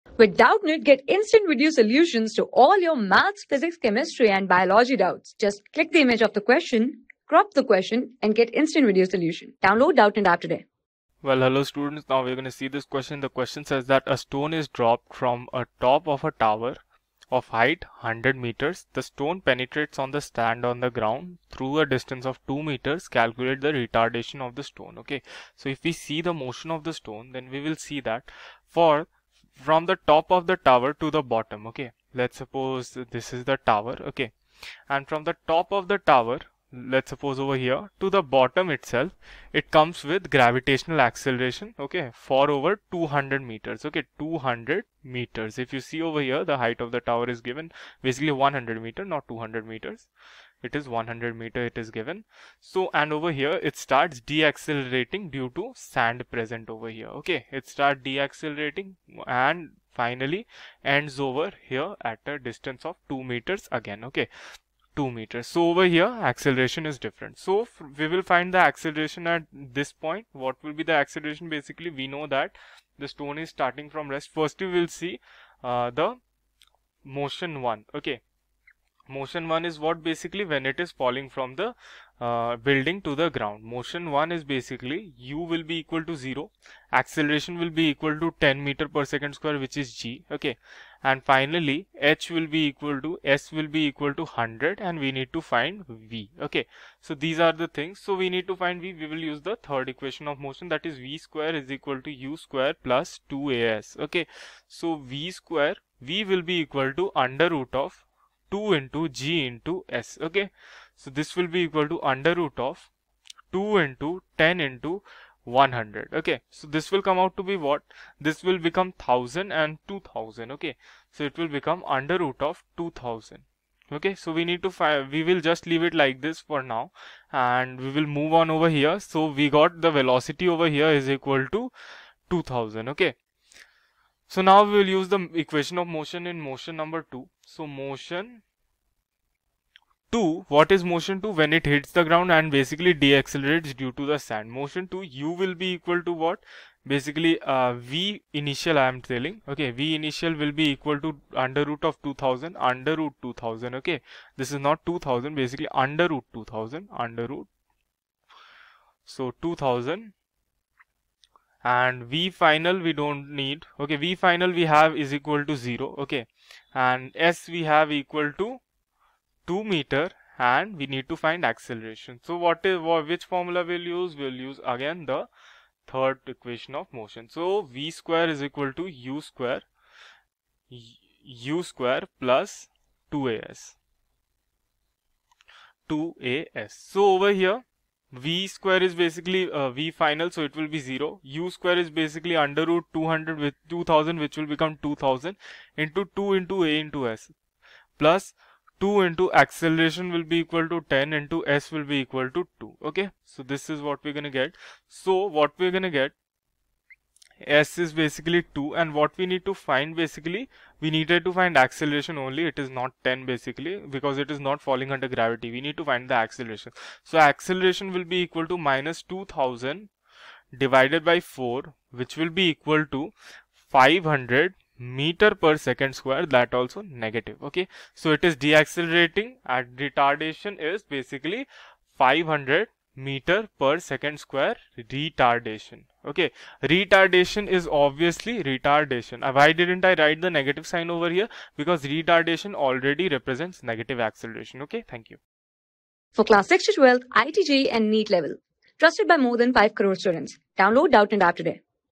A stone is dropped from the top of a tower of height 100 m. The stone penetrates in the sand on ...
A stone is dropped from the top of a tower of height 100 m. The stone penetrates in the sand on the ground through a distance of 2m. Calculate the retardation of the stone.
Class: 12
Subject: PHYSICS
Chapter: Motion in Straight Line
Board:IIT JEE

👉 Have Any Query? Ask Us.